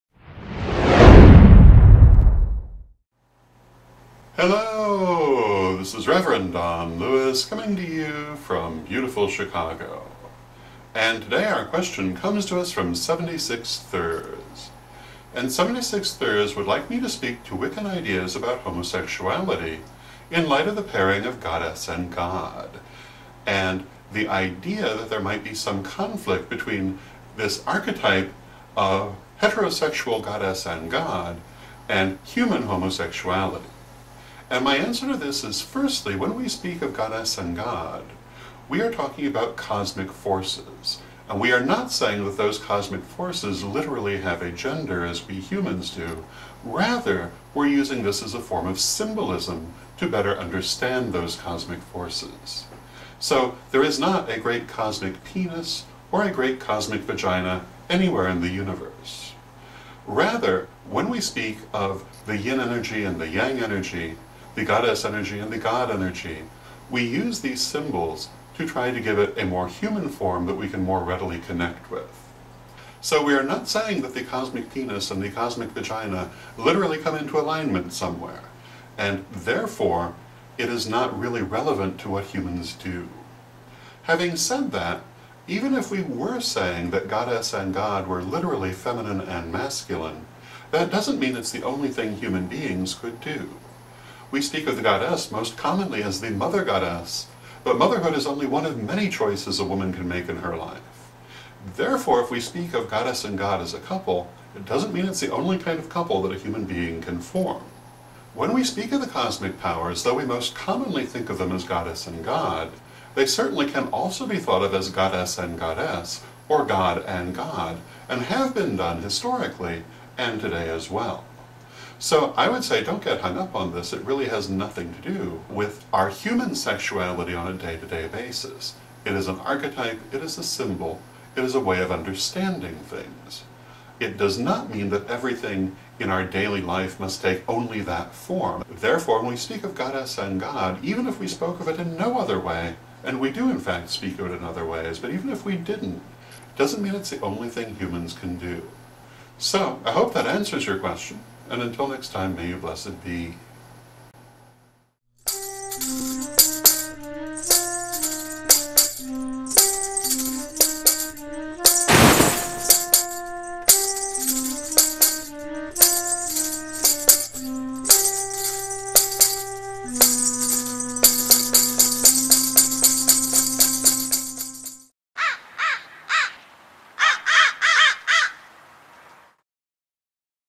Rev. Don's Vlog - Homosexuality
Rev. Don discusses Wiccan views on homosexuality in relation to the Goddess/God paradigm, in response to a question from 76Thurs. Like Rev. Don's daily Vlogs? Listen to Rev. Don Thursday nights at 9 PM Central/10 PM Eastern on http